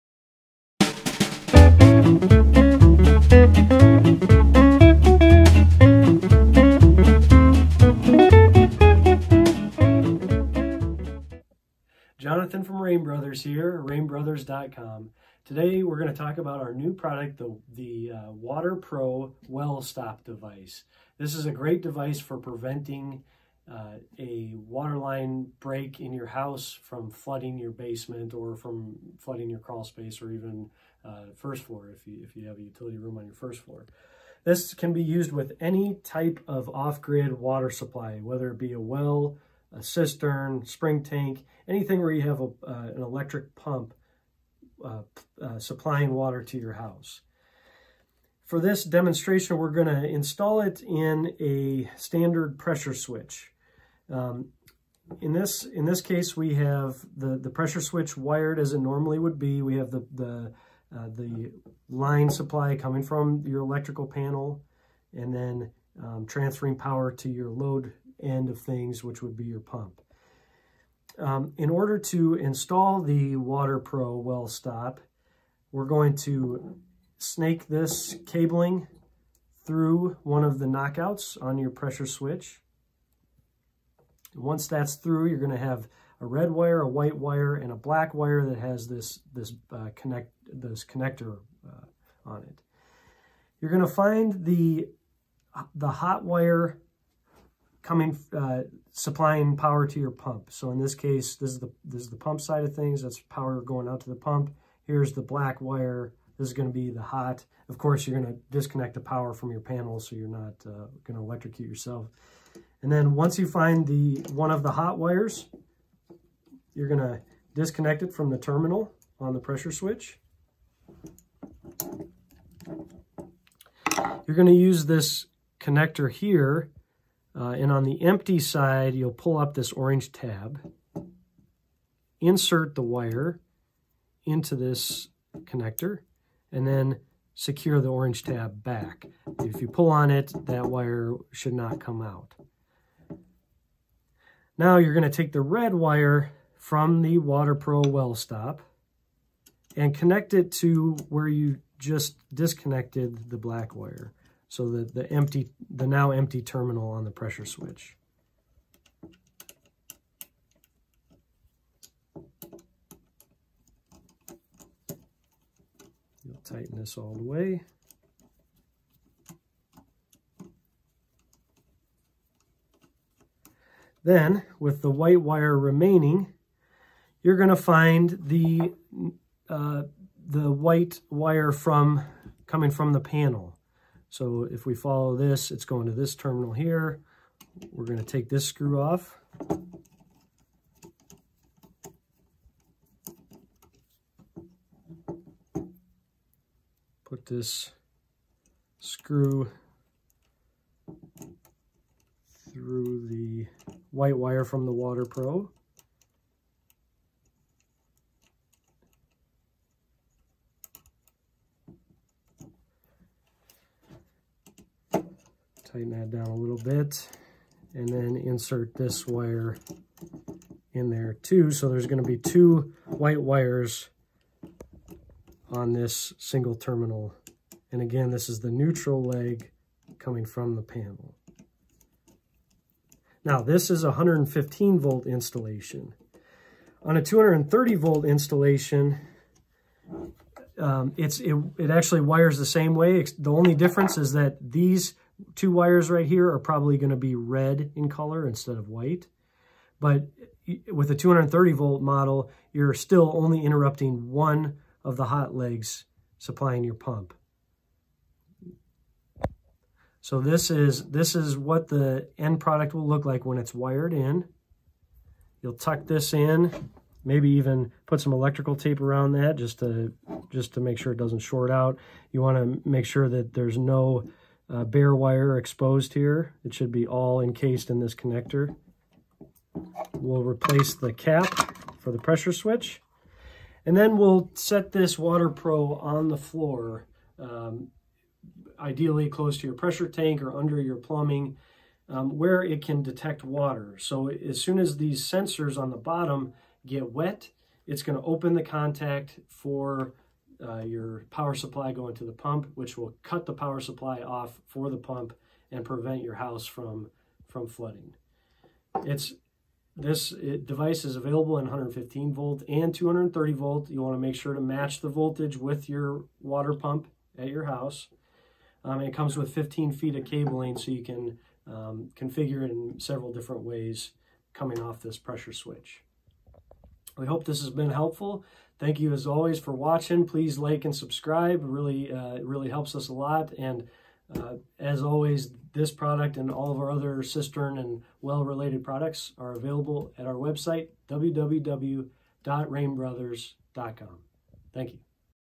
Prevent your house from flooding! Well pump shut-off device for pipe leaks
Here is an effective, easy way to add peace of mind in your home for those of us who use a well pump or cistern pump to supply water to our homes.